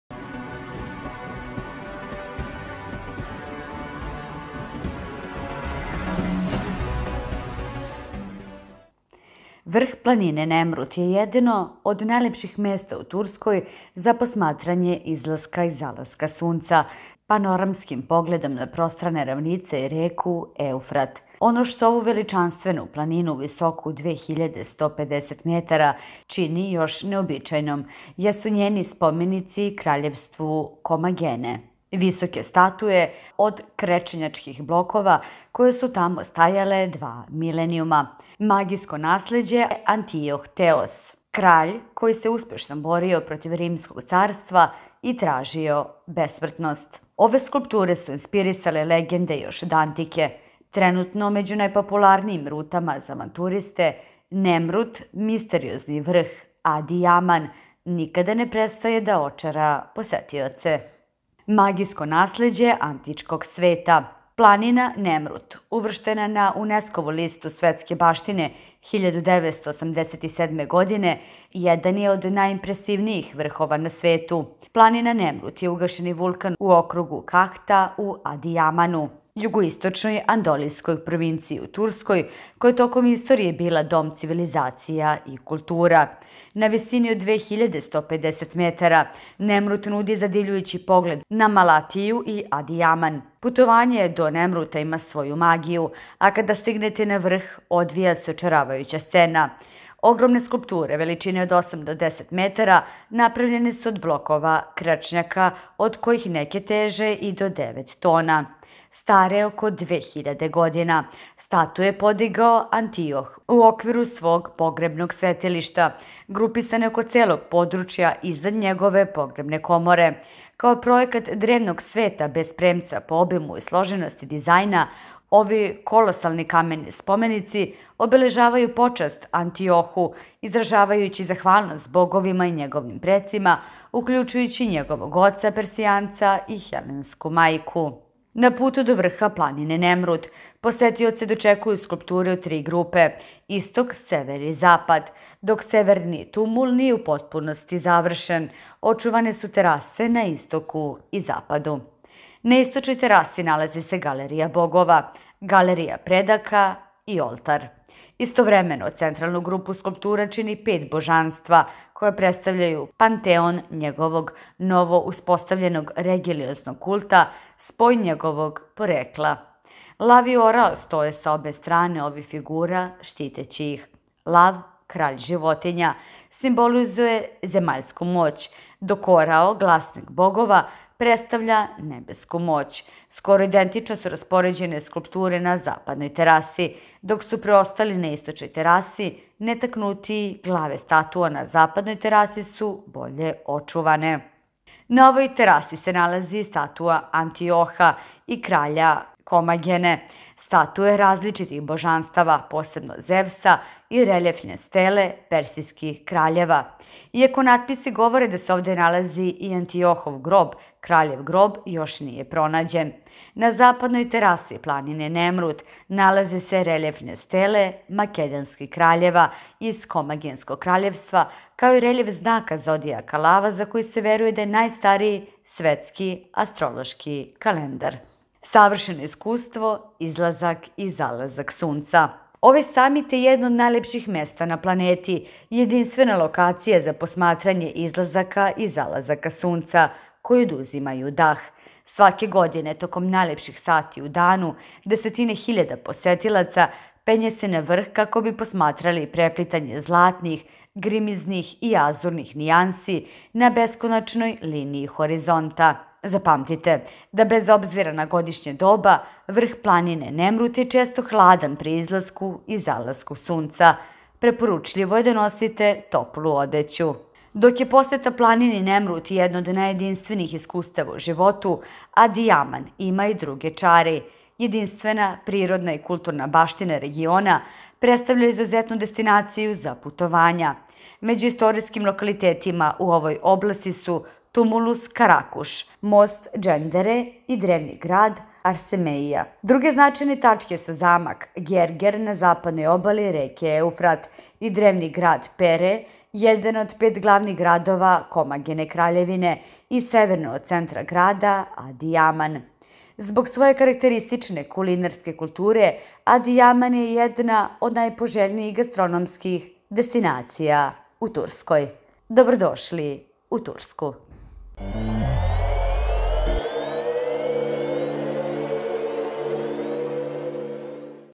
TURSKA planina NEMRUT
Vrh planine Nemrut je jedno od najlepših mesta u Turskoj za posmatranje izlaska i zalaska sunca, sa panoramskim pogledom na prostrane ravnice i reku Eufrat. Ono što ovu veličanstvenu planinu visoku 2.150 metara čini još neobičnijom jesu njeni spomenici kraljevstvu Komagene – visoke statue od krečnjačkih blokova koje su tamo stajale dva milenijuma. Podigao ih je Antioh Teos I (69-36 p.n.e.), kralj koji se uspešno borio protiv Rimskog carstva i tražio besmrtnost. Ove skulpture su inspirisale legende još od antike. Trenutno među najpopularnijim rutama za avanturiste, Nemrut, misteriozni vrh Adijaman, nikada ne prestaje da očara posetioce.
     Magijsko nasleđe antičkog sveta. Planina Nemrut, uvrštena na Uneskovu listu svetske baštine 1987. godine, jedan je od najimpresivnijih vrhova na svetu. Planina Nemrut je ugašeni vulkan u okrugu Kahta u Adijamanu, jugoistočnoj anadolijskoj provinciji u Turskoj koja je tokom istorije bila dom civilizacija i kultura. Na visini od 2150 metara, Nemrut nudi zadivljujući pogled na Malatiju i Adijaman. Putovanje do Nemruta ima svoju magiju, a kada stignete na vrh, odvija se očaravajuća scena. Ogromne skulpture veličine od osam do 10 metara napravljene su od blokova krečnjaka, od kojih neke teže i do devet tona. Stare oko dve hiljade godina, statue je podigao Antioh u okviru svog pogrebnog svetilišta, grupisane oko celog područja iznad njegove pogrebne komore. Kao projekat drevnog sveta bez premca po obimu i složenosti dizajna, ovi kolosalni kameni spomenici obeležavaju počast Antiohu, izražavajući zahvalnost bogovima i njegovim precima, uključujući  i njegovog oca Persijanca i helensku majku.
    Na putu do vrha planine Nemrut, posetioce dočekuju skulpture u tri grupe: istok, sever i zapad. Dok severni tumul nije u potpunosti završen, očuvane su terase na istoku i zapadu. Na istočnoj terasi nalaze se Galerija bogova, Galerija predaka i oltar. Istovremeno, centralnu grupu skulptura čini pet božanstava koja predstavljaju panteon njegovog novouspostavljenog religioznog kulta, spoj njegovog porekla. Lav i orao stoje sa obe strane ovih figura, štiteći ih. Lav, kralj životinja, simbolizuje zemaljsku moć, dok orao, glasnik bogova, predstavlja nebesku moć. Skoro identično su raspoređene skulpture na zapadnoj terasi. Dok su prestoli na istočnoj terasi netaknutiji, glave statua na zapadnoj terasi su bolje očuvane. Na ovoj terasi se nalazi i statua Antioha I, kralja Komagene, statue različitih božanstava, posebno Zevsa, i reljefne stele persijskih kraljeva. Iako natpisi govore da se ovde nalazi i Antiohov grob, kraljev grob još nije pronađen. Na zapadnoj terasi planine Nemrut nalaze se reljefne stele makedonskih kraljeva iz Komagenskog kraljevstva, kao i reljef znaka zodijaka lava, za koji se veruje da je najstariji svetski astrološki kalendar.
    Savršeno iskustvo: izlazak i zalazak sunca. Ovaj samit je jedno od najlepših mesta na planeti i jedinstvena lokacija za posmatranje izlazaka i zalazaka sunca koji oduzimaju dah. Svake godine, tokom najlepših sati u danu, desetine hiljada posetilaca penje se na vrh kako bi posmatrali preplitanje zlatnih, grimiznih i azurnih nijansi na beskonačnoj liniji horizonta. Zapamtite da bez obzira na godišnje doba, vrh planine Nemrut je često hladan pri izlasku i zalasku sunca – preporučljivo je da nosite toplu odeću.
     Više od Adijamana.Dok je poseta planini Nemrut jedno od najjedinstvenijih iskustava u životu, Adijaman ima i druge čari. Jedinstvena prirodna i kulturna baština regiona predstavlja izuzetnu destinaciju za putovanja. Među istorijskim lokalitetima u ovoj oblasti su tumulus Karakuš, most Džendere i drevni grad Arsemeia. Druge značajne tačke su zamak Gerger na zapadnoj obali reke Eufrat i drevni grad Pere, jedan od pet glavnih gradova Komagene kraljevine i severno od centra grada Adijaman. Zbog svoje karakteristične kulinarske kulture, Adijaman je jedna od najpoželjnijih gastronomskih destinacija u Turskoj.